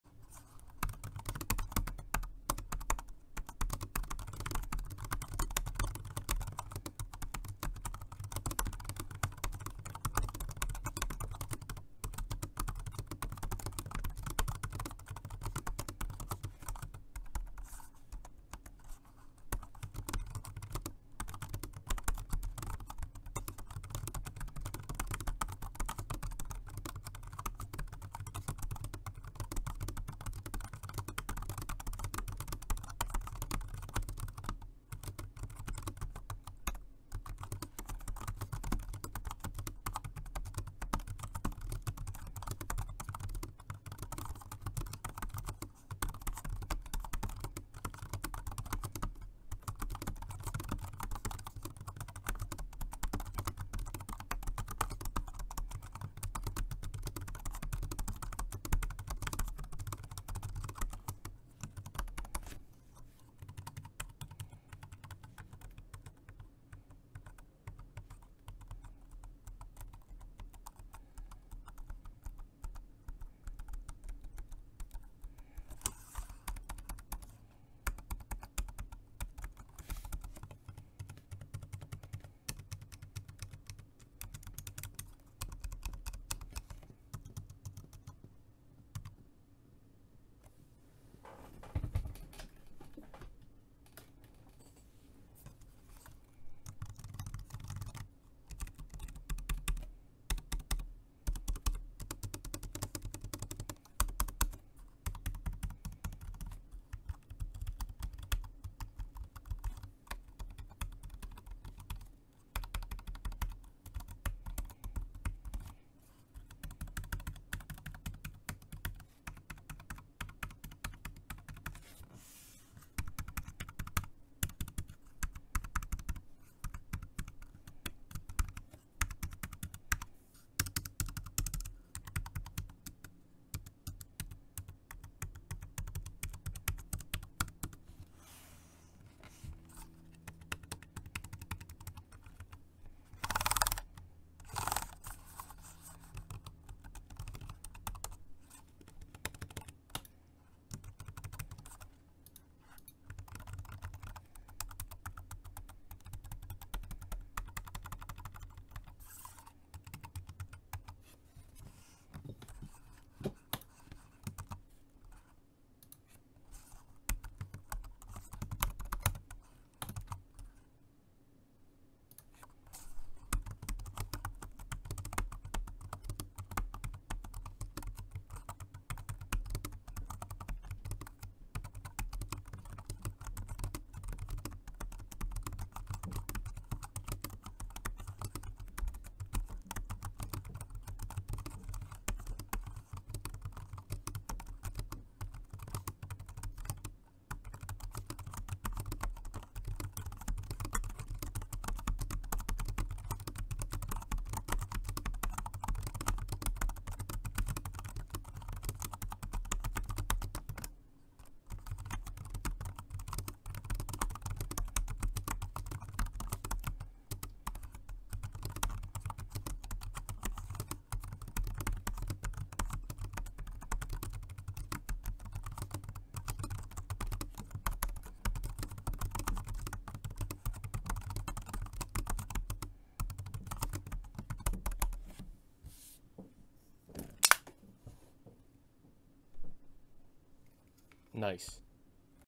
[ASMR] Multi-Device Keyboard Jelly Comb Dual Mode Rechargeable 2.4G Wireless & Bluetooth Typing Test
Video: Google Pixel

Audio: Zoom H5 (Set at level 7)